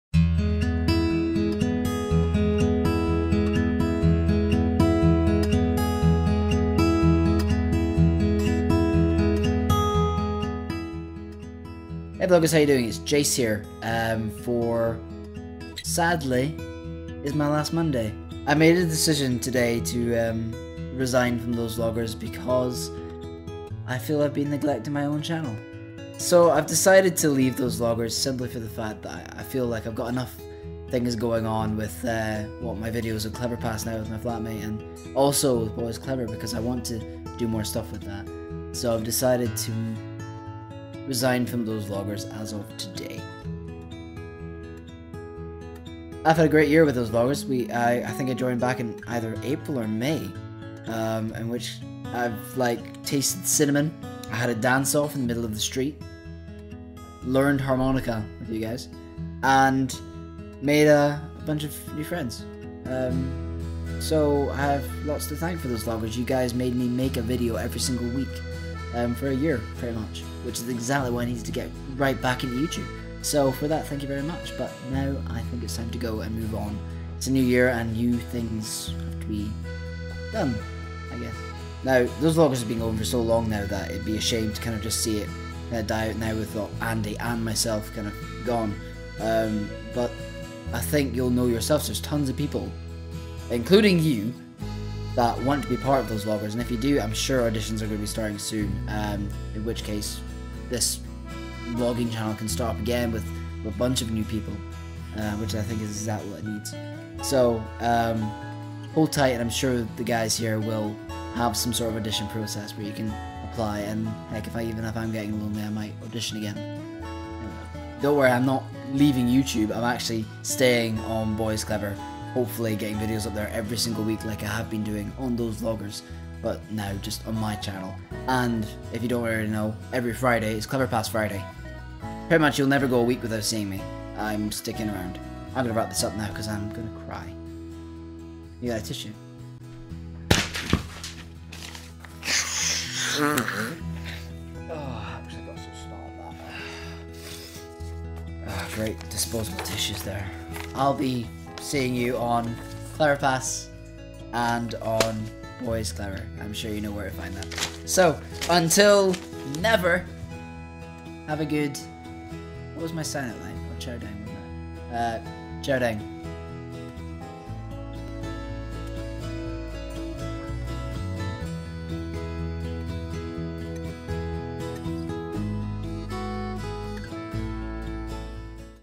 168 Jase leaves Thosevloggers =(
Sorry to say it but I've decided to call it a day so I can concentrate on other things!

50 likes and I'll consider staying =P

You've all been so great but you can still catch me on my other two channels regularly!